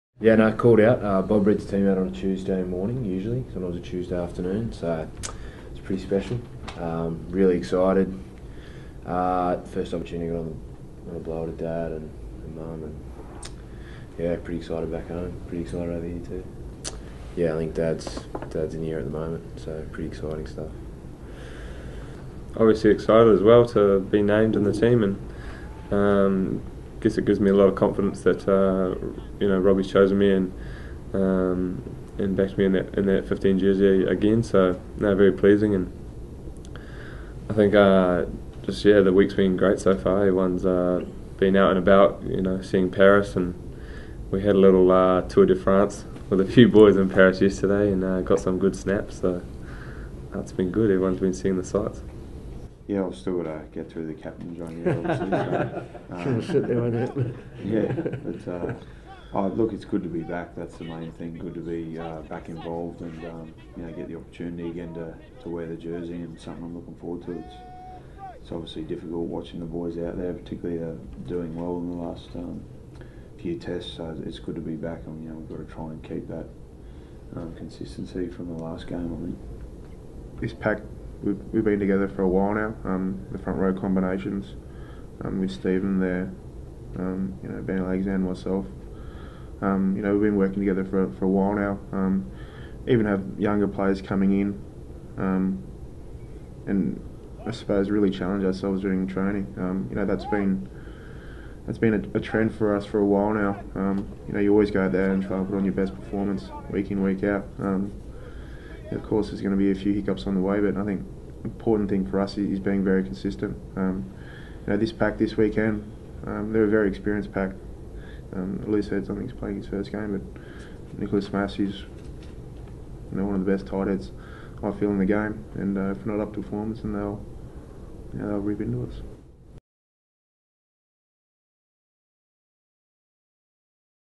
Spring Tour 2012 - Wallabies team naming v France
Qantas Wallabies Paddy Ryan, Stephen Moore and Mike Harris talk to the media ahead of their Test against France on Spring Tour 2012.